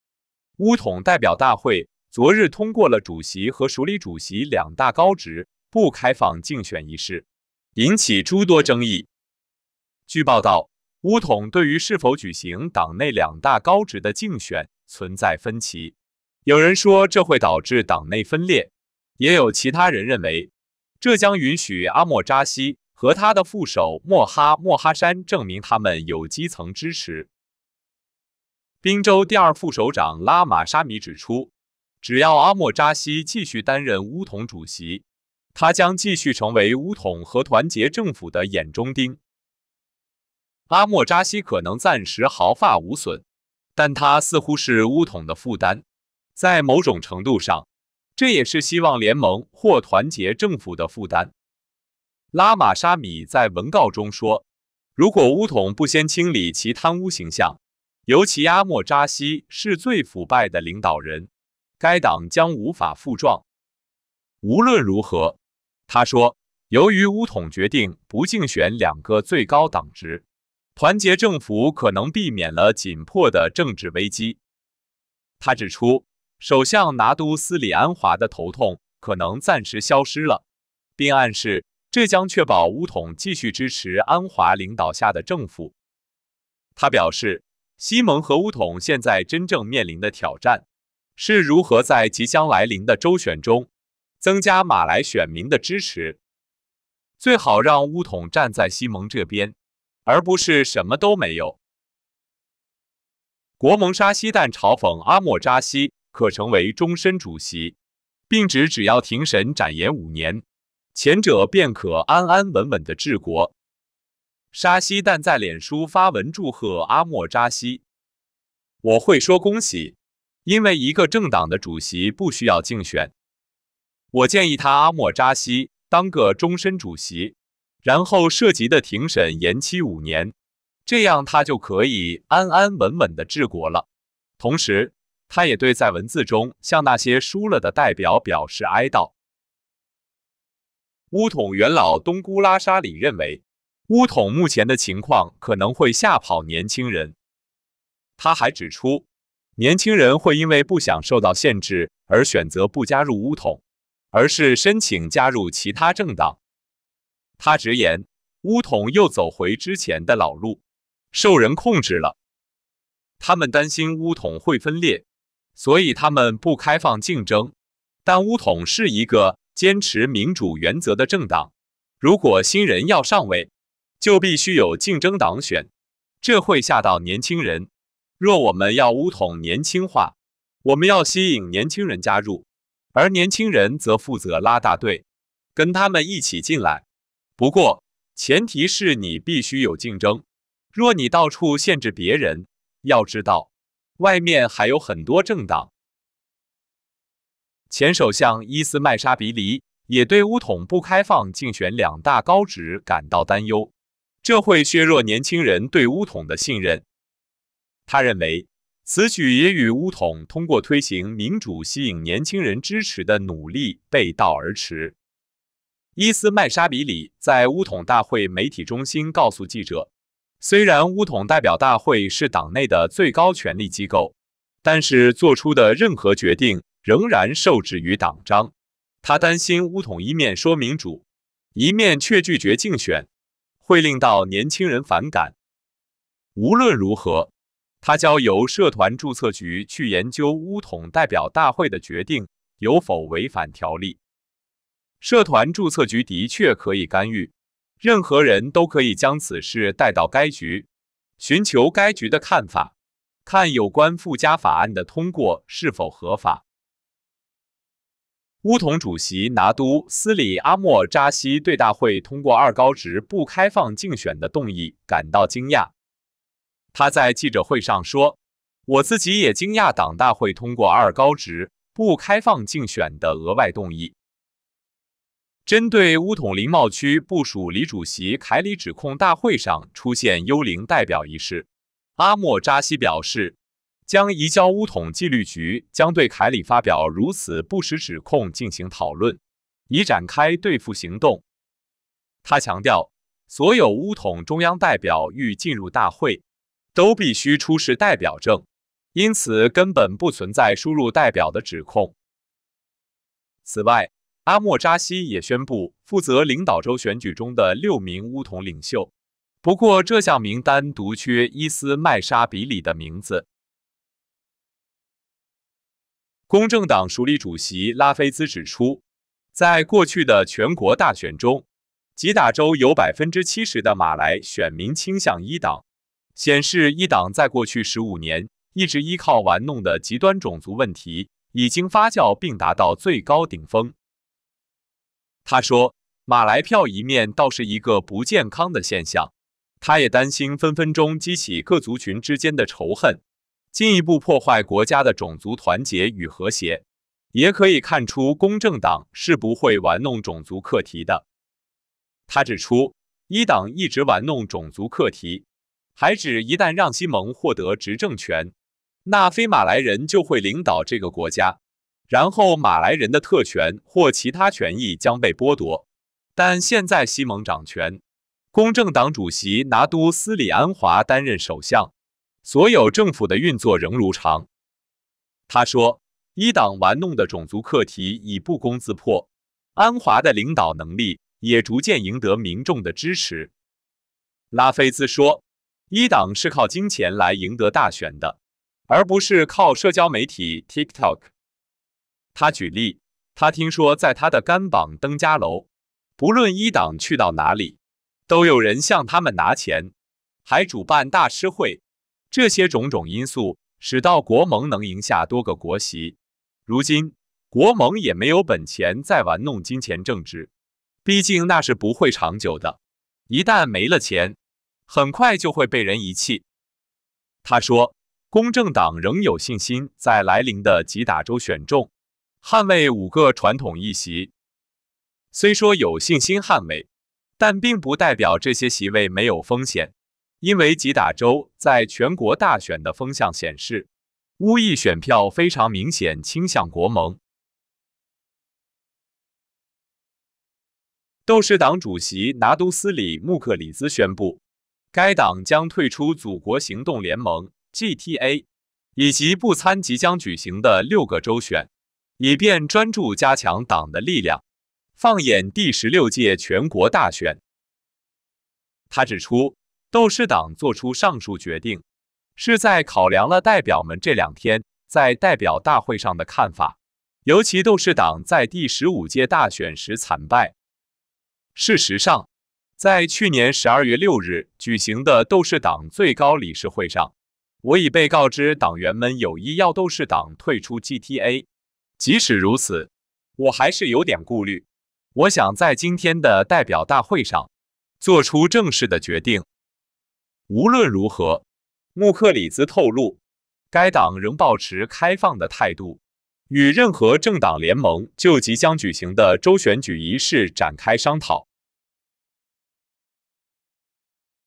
阿末扎希一揽独大！团结政府稳了？拉玛沙米说，虽然安华解决了头痛问题，不过阿末扎希将继续成为巫统和团结政府的“眼中钉”。
巫统代表大会昨日通过了主席和署理主席两大高职不开放竞选一事，引起诸多争议。